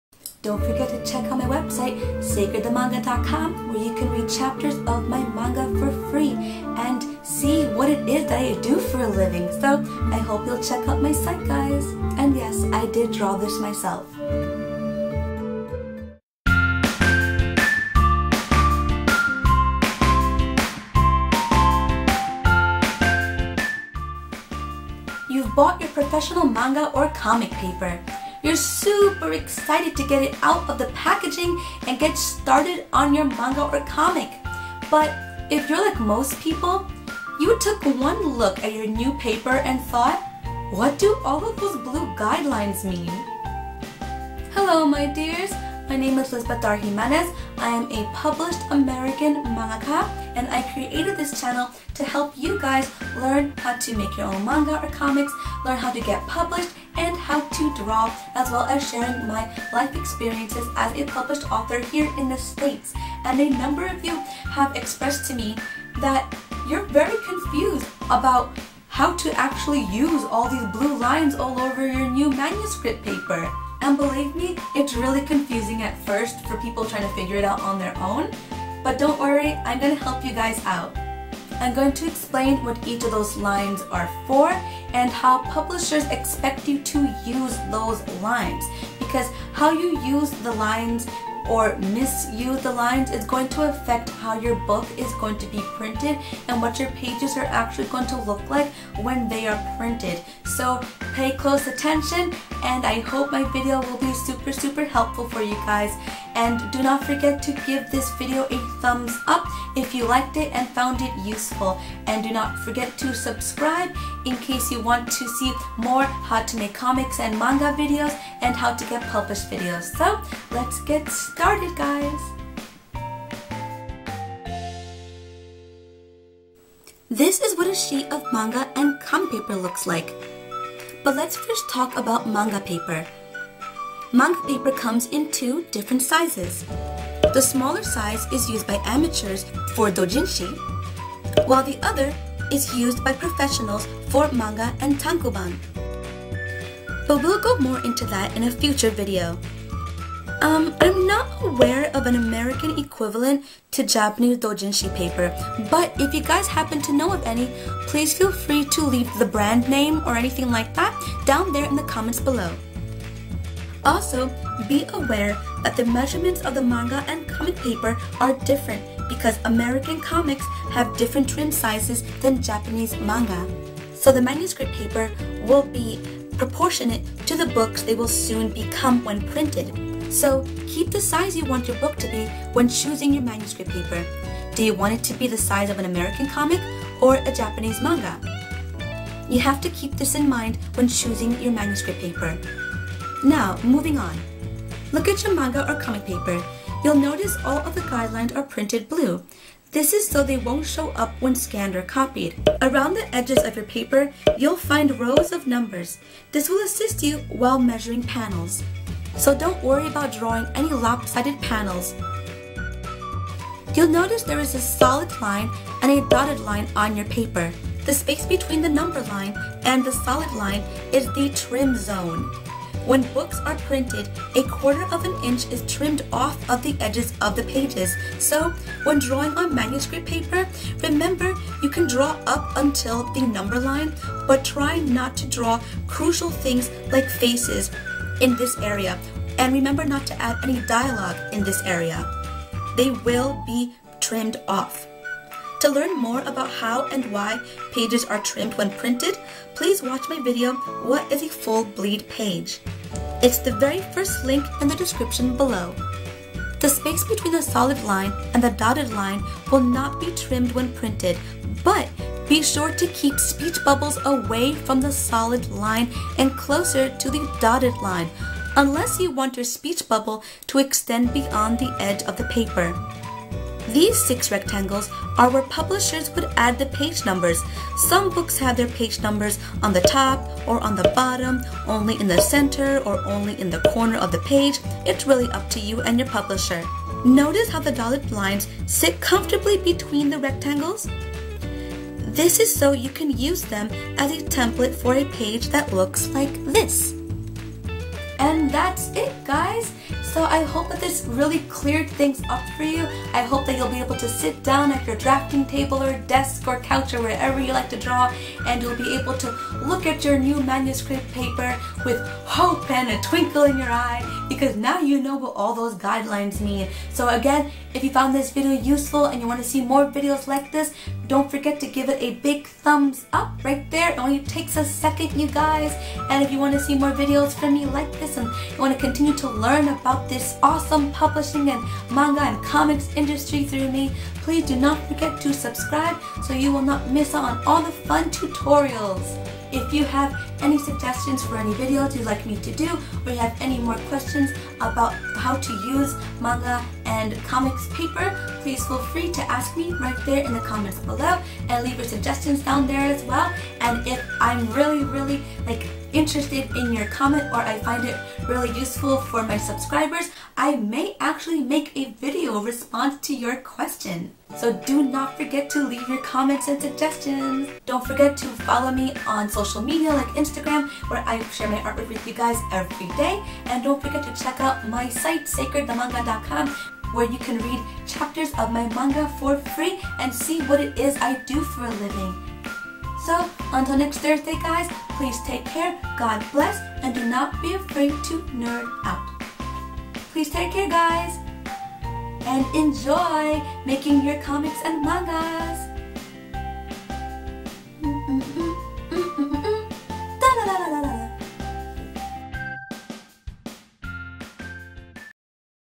❤ Manga/Comic Paper ❤ What do the Blue Guide Lines Mean & How to Use Them
🔥🔥▼▼MY "HOW TO DRAW MANGA" BOOK▼▼🔥🔥
Book comes with manga paper, dipping pens, & MORE!

Hello dear ones! I'll be showing you how to properly use the blue guidelines on your manga or comic paper and tell you why using them properly will be beneficial when you get your work published!

-"What do those blue lines on manga paper/comic paper/ manuscript paper mean?"

-" What are those blue lines for?"